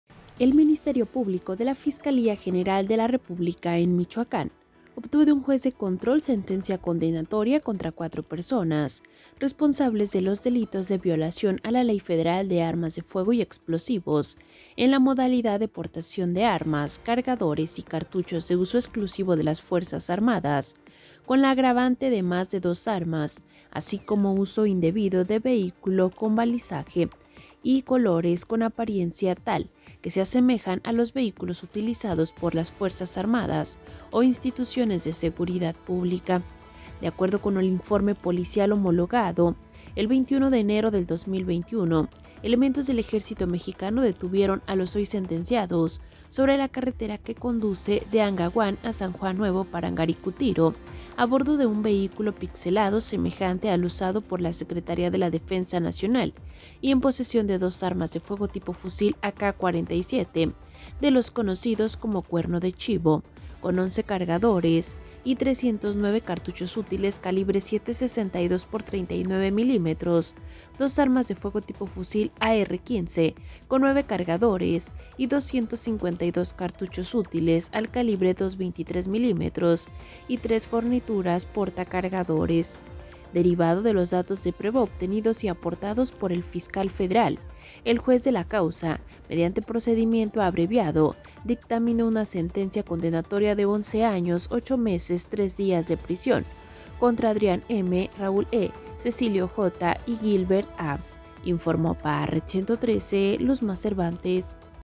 Casi 12 años de cárcel a 4 que fueron detenidos con arsenal y camión “pirata” de Sedena
San Juan Nuevo Parangaricutiro, Mich.- 23 de agosto de 2023.- El Ministerio Público de la Fiscalía General de la República (FGR) en Michoacán, obtuvo de un juez de control Sentencia Condenatoria contra cuatro personas,responsables de los delitos de violación a la Ley Federal de Armas de Fuego y Explosivos en la modalidad de portación de armas, cargadores y cartuchos de uso exclusivo de las Fuerzas Armadas, con la agravante de más de dos armas, así como Uso Indebido de Vehículo con Balizaje y Colores con Apariencia tal que se Asemejen a los Vehículos Utilizados por las Fuerzas Armadas o Instituciones de Seguridad Pública.

De acuerdo con el Informe Policía Homologado (IPH), el 21 de enero del 2021, elementos del Ejército Mexicano detuvieron a los hoy sentenciados sobre la carretera que conduce de Angahuan a San Juan Nuevo Parangaricutiro, a bordo de un vehículo pixelado semejante al usando por la Secretaría de la Defensa Nacional y  en posesión de dos armas de fuego tipo fusil AK-47 de los conocidos como “cuerno de chivo” con once cargadores y 309 cartuchos útiles al calibre 7.62 x 39  milímetros, dos armas de fuego tipo fusil AR-15 con nueve cargadores y 252 cartuchos útiles al calibre .223 milímetros y tres fornituras porta cargadores.

Derivado de los datos de prueba obtenidos y aportados por el Fiscal Federal, el Juez de la Causa, mediante procedimiento abreviado, dictaminó una sentencia condenatoria de once años ocho meses tres días de prisión, contra Adrián “M”, Raúl “E”, Cecilio “J” y Wilber “A”.